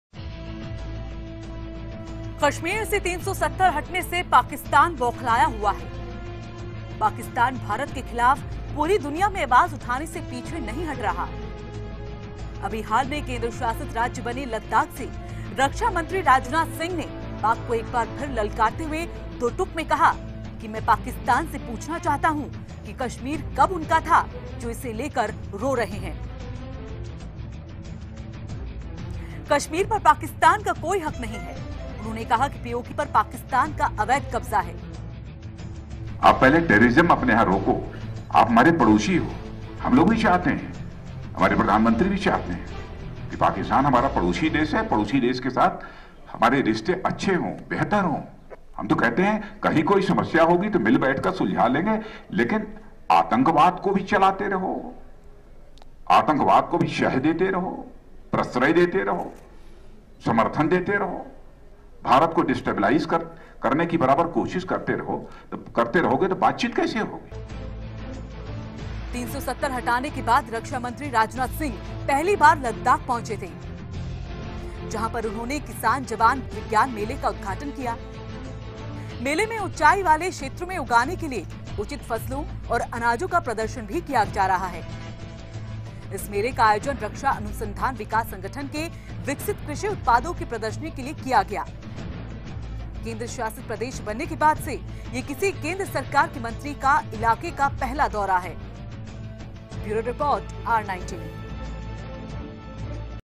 पाक को रक्षामंत्री राजनाथ सिंह की दो टूक
राजनाथ सिंह ने पाक को लिया आड़े हाथ... ‘कश्मीर कब पाकिस्तान का हिस्सा था, जो रो रहे हो’… पाकिस्तान बन गया तो हम आपके वजूद का सम्मान करते हैं.